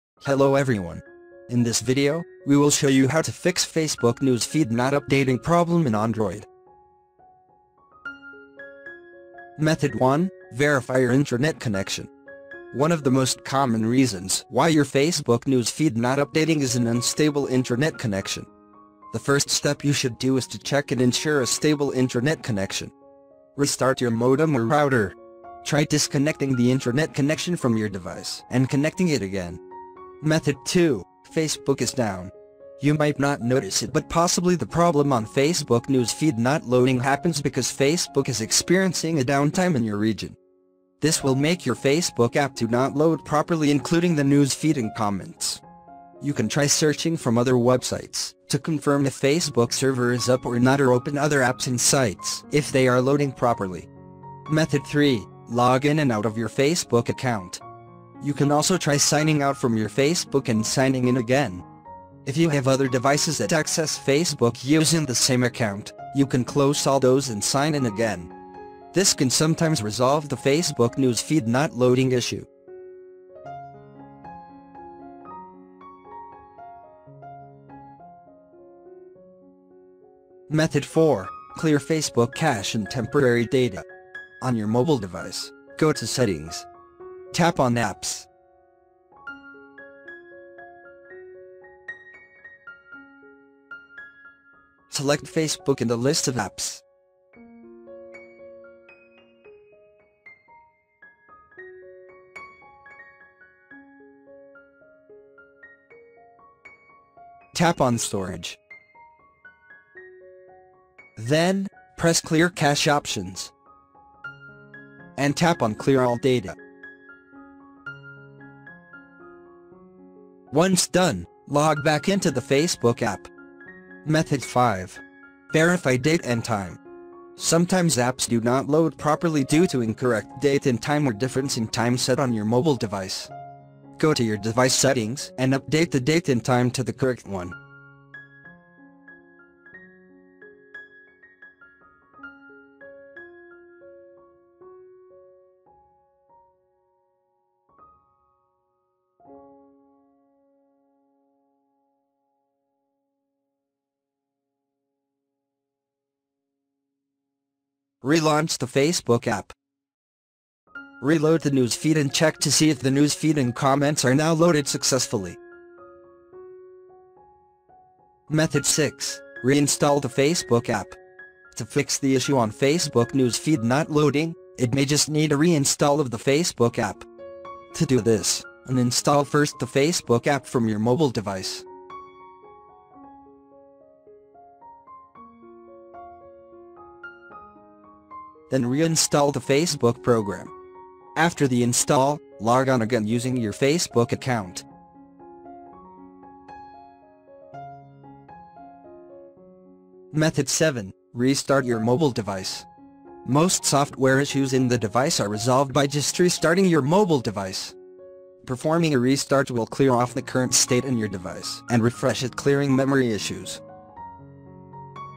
How to Fix Facebook News feed Not Showing Problem in Android
In this video, we will show you how to fix Facebook news feed not updating problem in Android.
Method 1: Verify Your Internet Connection.
Method 2: Facebook is Down.
Method 3: Log in and out of your Facebook Account.
Method 4: Clear Facebook Cache and Temporary Data.
Method 5. Verify Date and Time.
Method 6: Reinstall the Facebook App
Method 7: Restart Your Mobile Device.